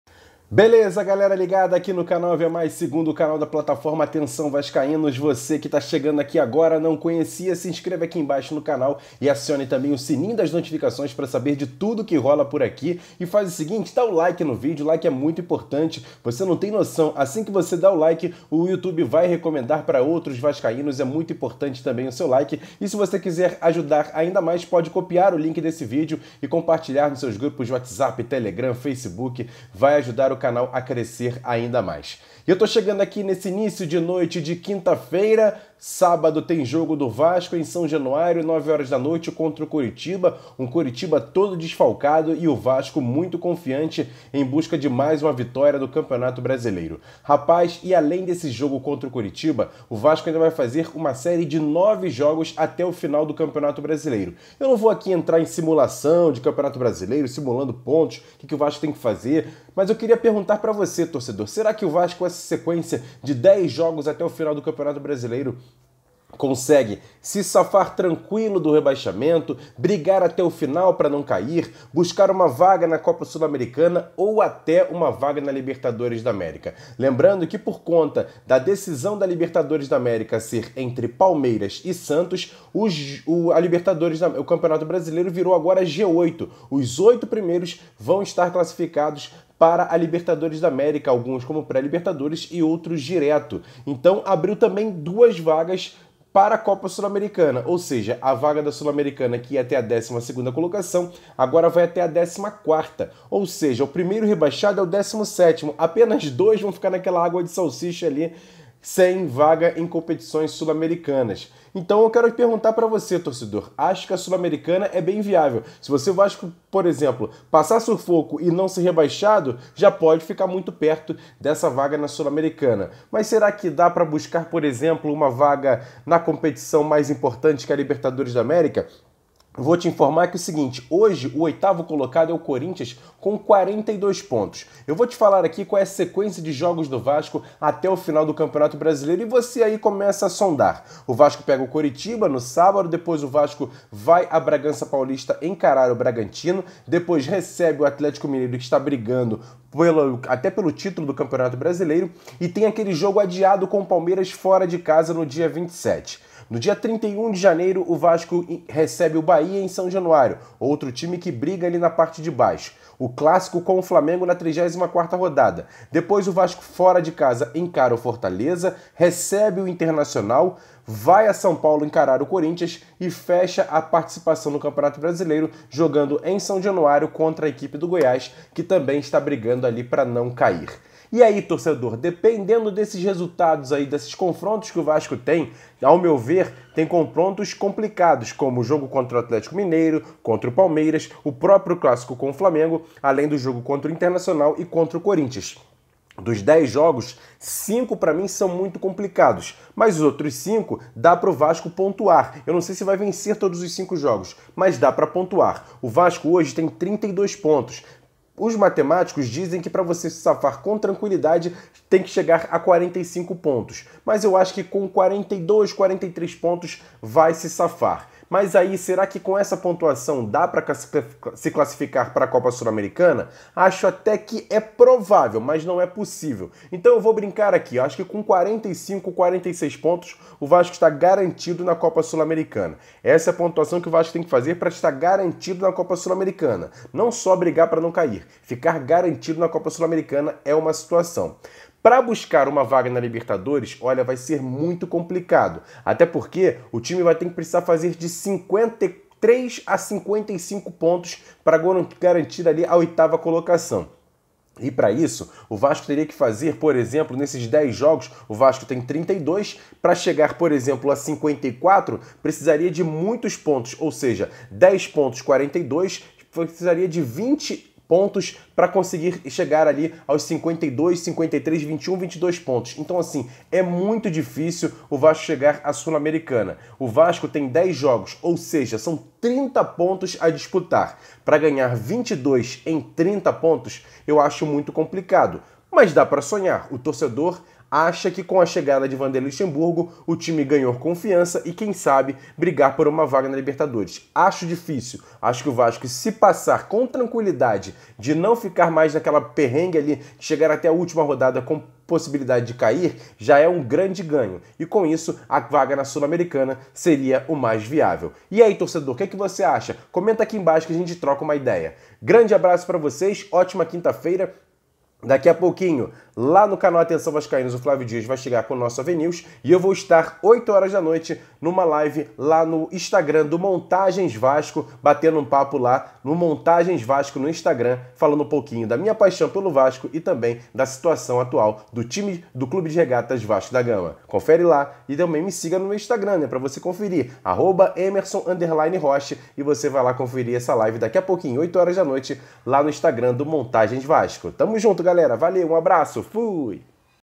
DÁ PARA SONHAR? SAIBA O QUE O VASCO TEM QUE FAZER PARA CHEGAR AS METAS NO BR-20. LIBERTA É POSSÍVEL?
SEJA MEMBRO DO CANAL ATENÇÃO, VASCAÍNOS!

ASSOCIA VASCO!